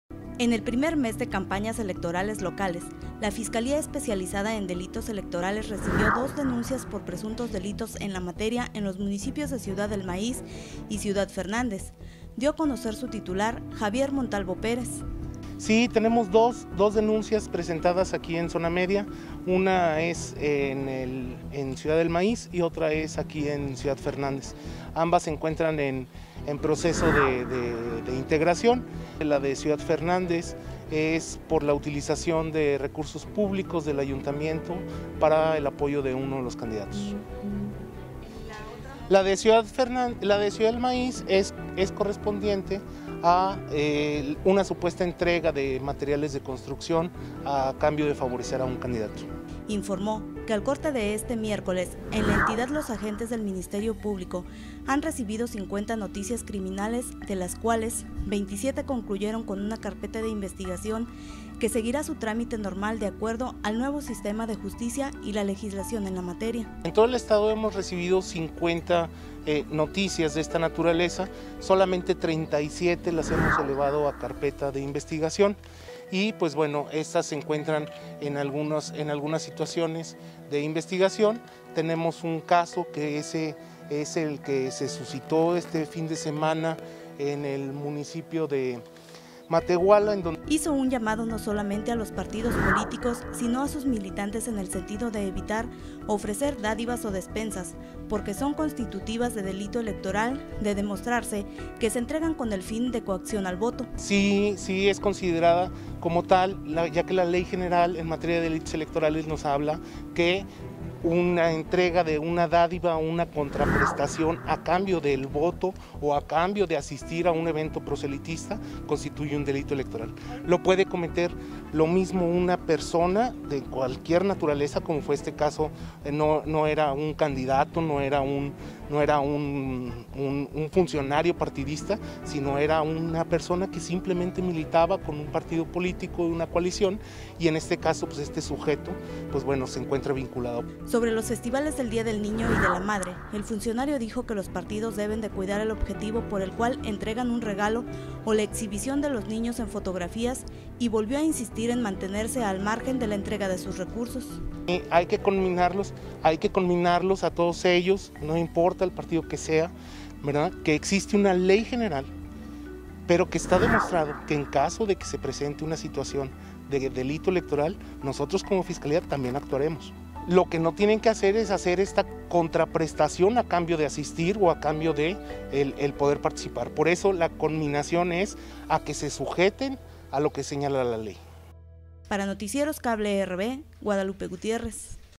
ACUMULAN DOS DELITOS ELECTORALES EN LA ZONA MEDIA
Hasta El Momento En Lo Que Va Del Proceso Electoral, En La Zona Media Se Han Recibido Dos Denuncias Por Delitos Electorales, En Ciudad Fernández Y Ciudad Del Maíz.

NOTICIEROS CABLE RV